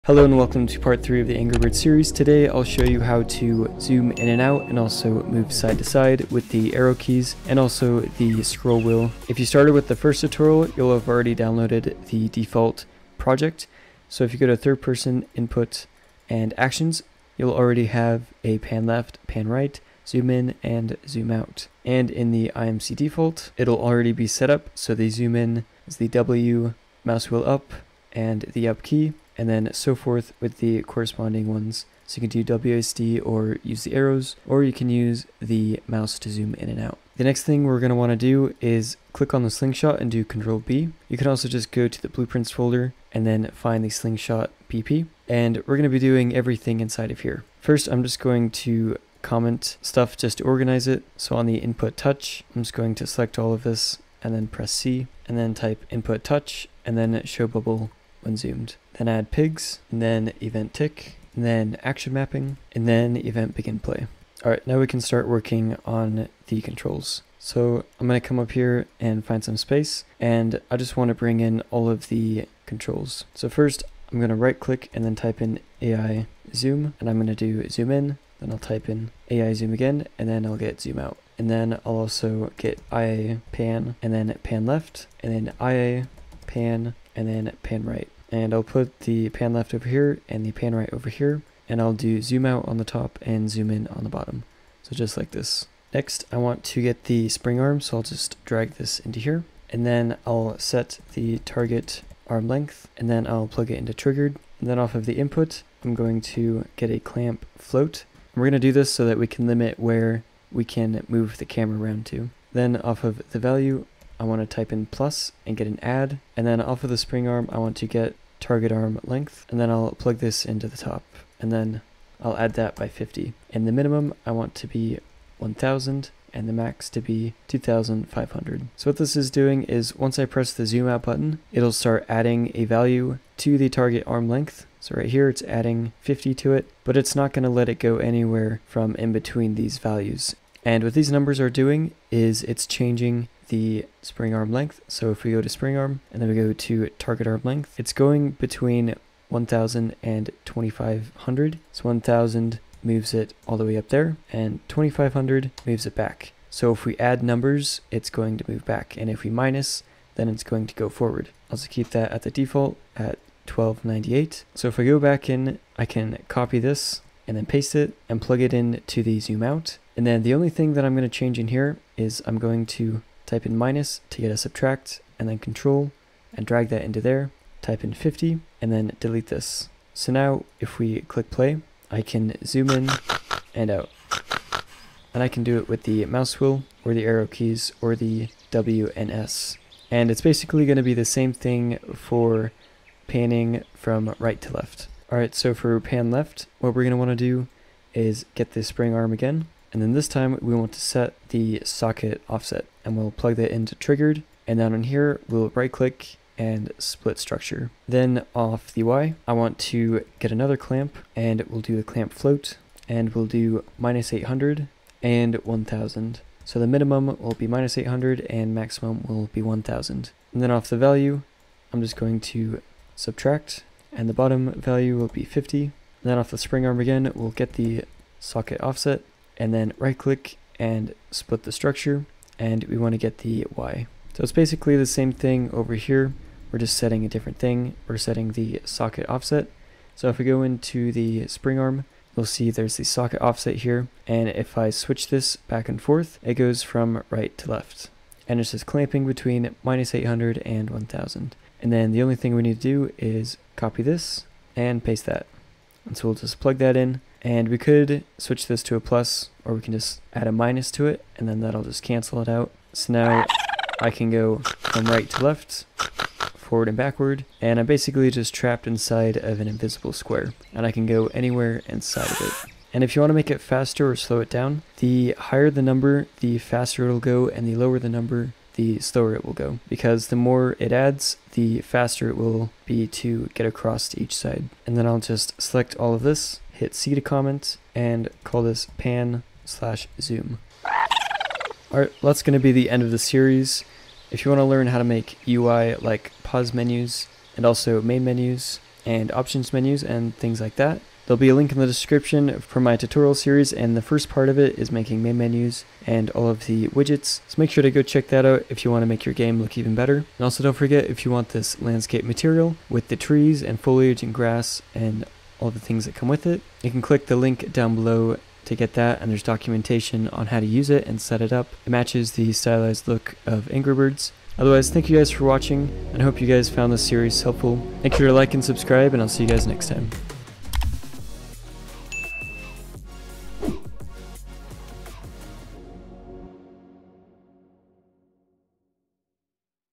How to make Angry Birds in Unreal Engine 5 | Part 3
Today you will learn how to use the arrow keys/mouse wheel to zoom in and out and pan around the level!

Tutorial Folder
Stylized Landscape Download Links